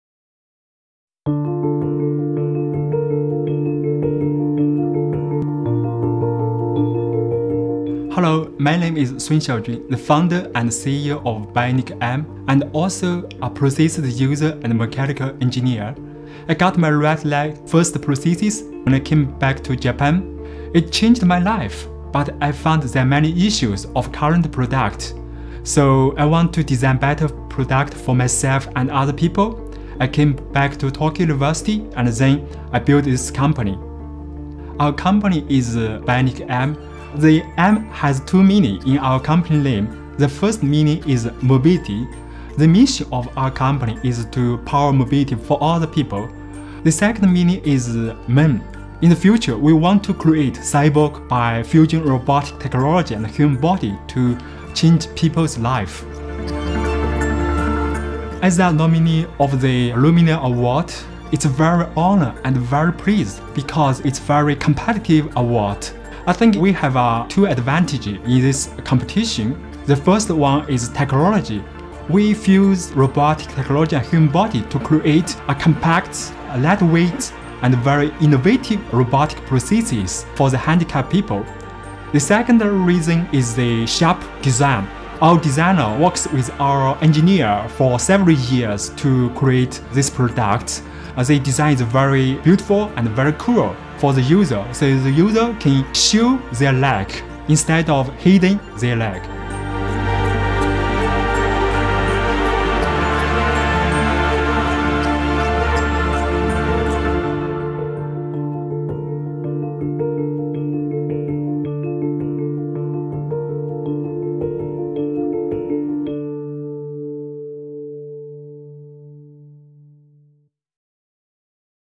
2020 Red Dot: Luminary Winner - Robotic Prosthetic Knee
2020 Red Dot: Luminary Winner Robotic Prosthetic Knee. The design concept is an innovative motor-powered prosthetic knee. It is designed for above-knee amputees and helps to restore their physical functions. Users can conduct daily activities with reduced physical pain.

The Red Dot: Luminary is the highest level of recognition accorded at the Red Dot Award: Design Concept. It is selected among the Red Dot: Best of the Best winners. An interview with the CEO of BionicM Inc., Dr. Sun Xiaojun.